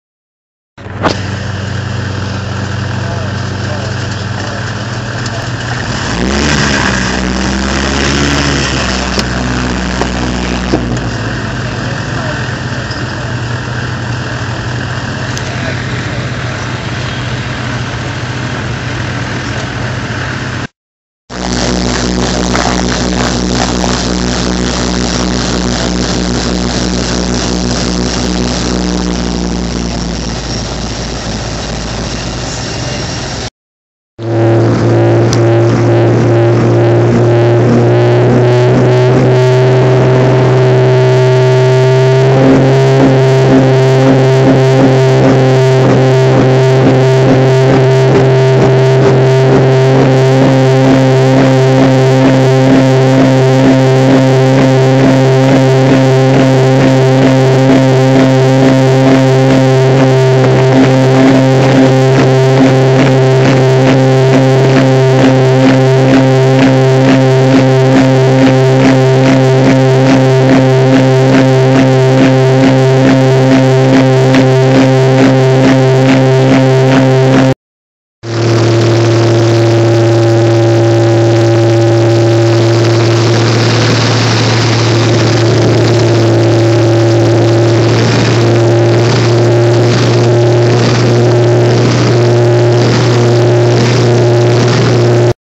Cape Air Movie
Flight from Boston to Provincetown on Cape Air